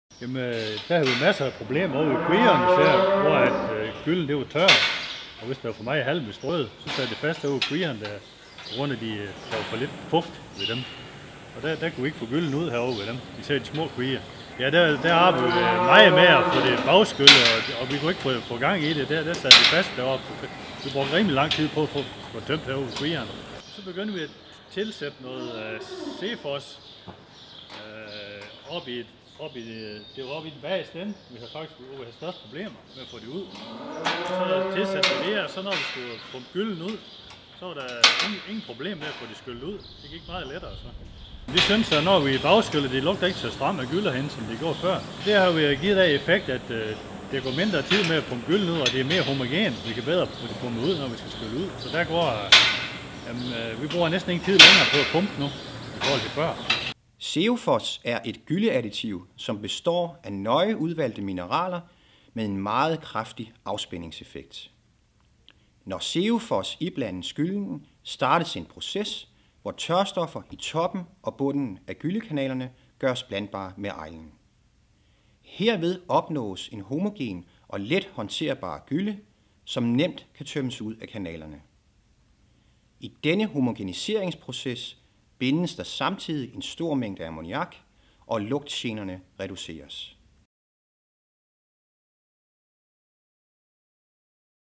Seofoss til Kvæg
SeoFoss er et gylleadditiv, som består af en nøje udvalgt blanding af specialmineraler, som giver en særdeles god ionbytteeffekt.
• Binder kvælstof i gyllen og giver dermed en bedre gødnings-værdi
• Gør gyllen homogen, hvilket dermed også tilfører afgrøderne en ensartet gødning
• Reducerer lugtgener ved at binde kvælstof i gyllen. Dette giver et bedre miljø både i stalden og i nærmiljøet til gavn for dyr og mennesker
• Giver dig arbejdsbesparelser, fordi gyllen bliver mere homogen og dermed lettere at håndtere.
• Let at anvende
• Må anvendes af økologer